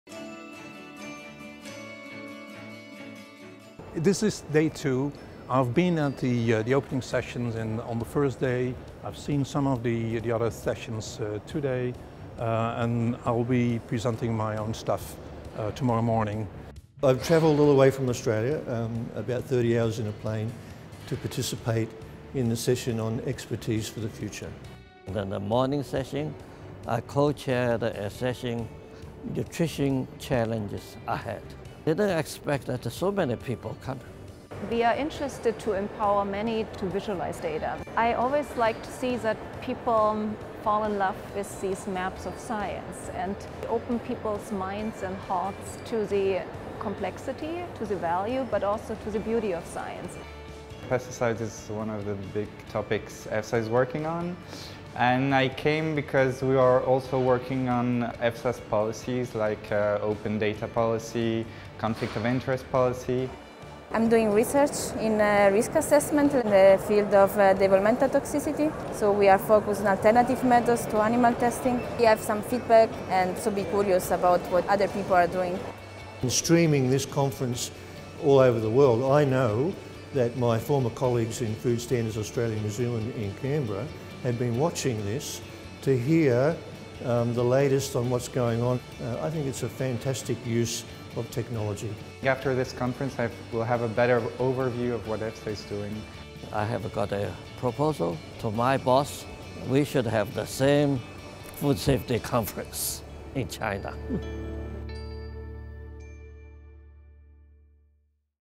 “Shaping the Future of Food Safety, Together”: highlights from day 2 of EFSA’s scientific conference
Hundreds of scientists, academics and policy-makers from around the world continued to debate the pressing issues facing food safety experts in the 21st century.